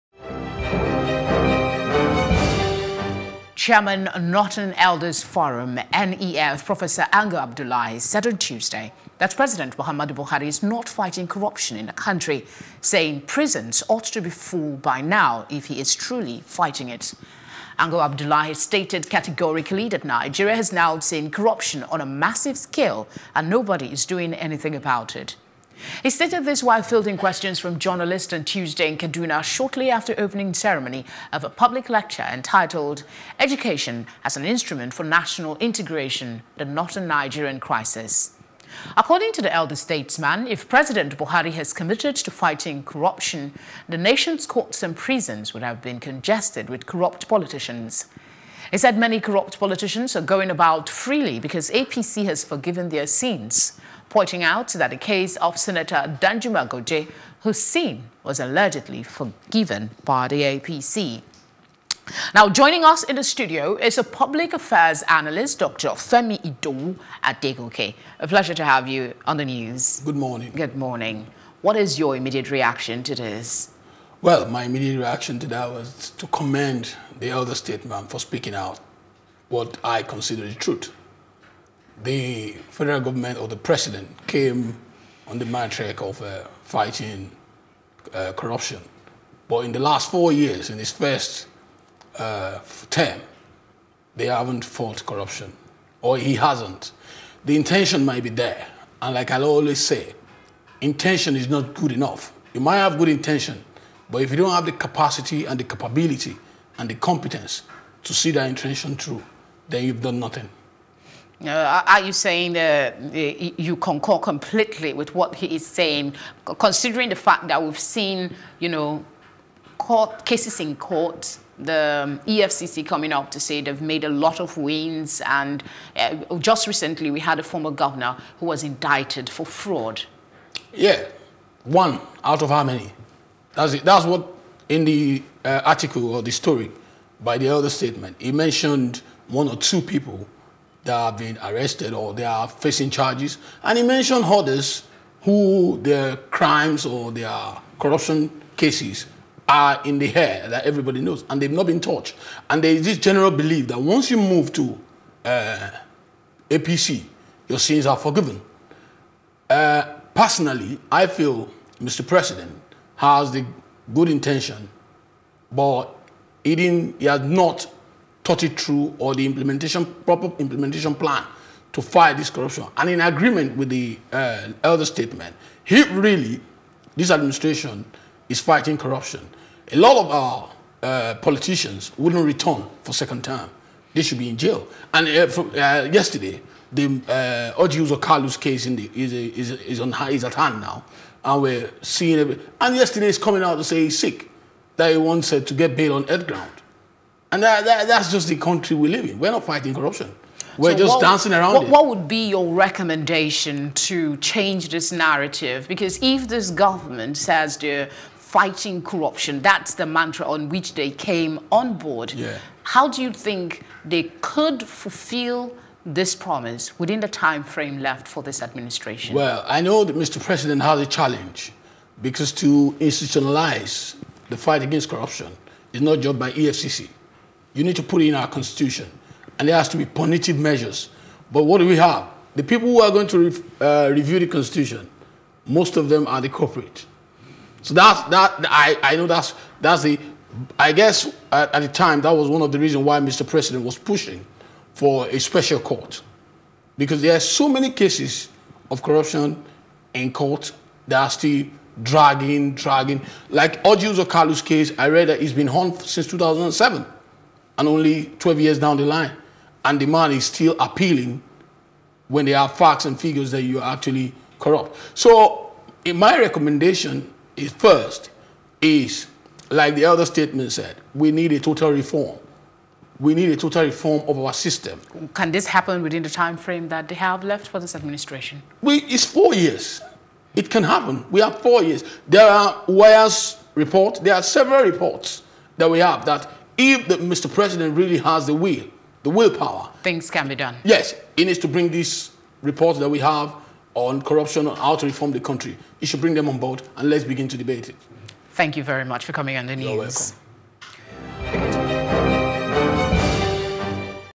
Anti-Graft War: Ango Abdullahi Says Buhari Not Fighting Corruption
Chairman, Northern Elders’ Forum, (NEF), Professor Ango Abdullahi said on Tuesday that President Muhammadu Buhari is not fighting corruption in the country, saying “prisons ought to be full by now, if he is truly fighting it.”